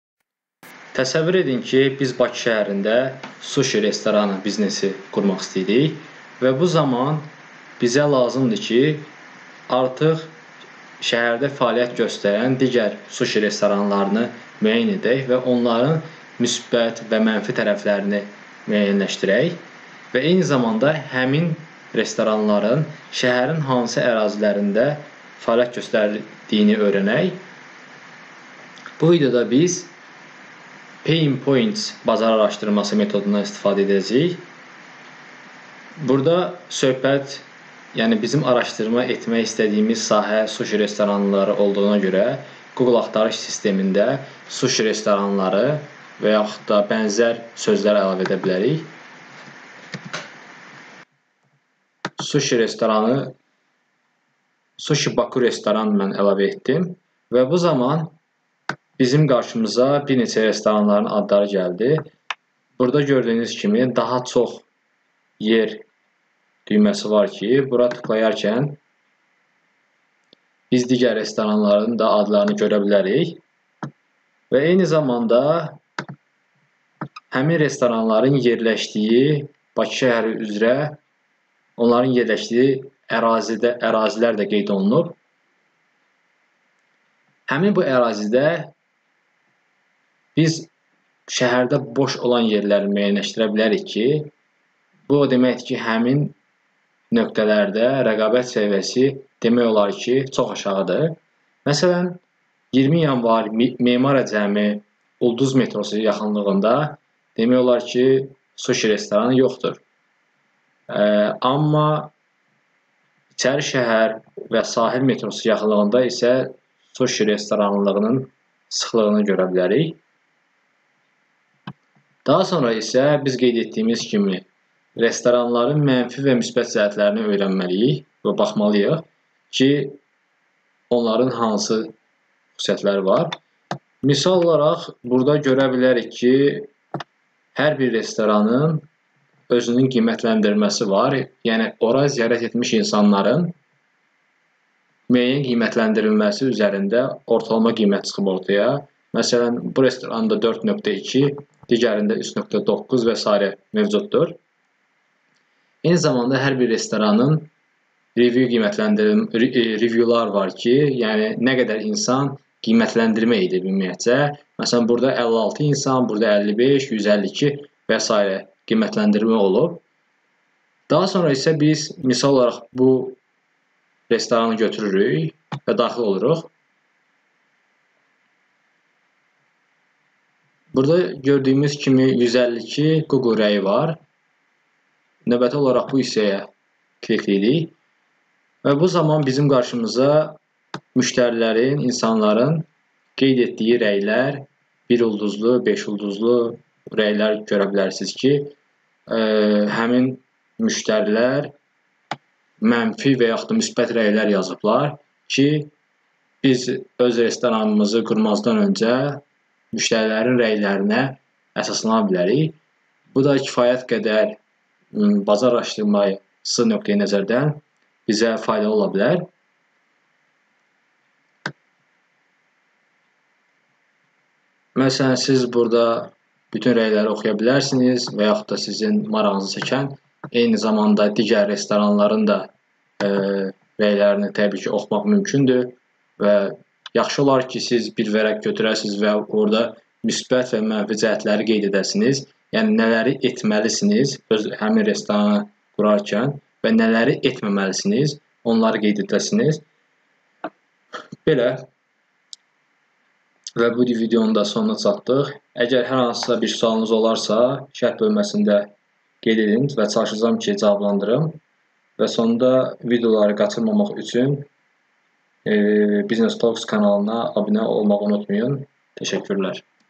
Bazar araşdırması | 1-ci hissə
Tutaq ki , biz Baki seherinde Sushi restorani biznesi qurmaq isteyirik ve bu zaman seherde fealiyyet gosteren diger sushi restoranlarini tehlil etmeliyik. Bu halda onlarin musbet ve menfi tereflerini arasdirmali ve ozumuze qeyd kimi goturmeliyik ki , yeni qurulacag restoran biznesi daha reqabetcil olsun .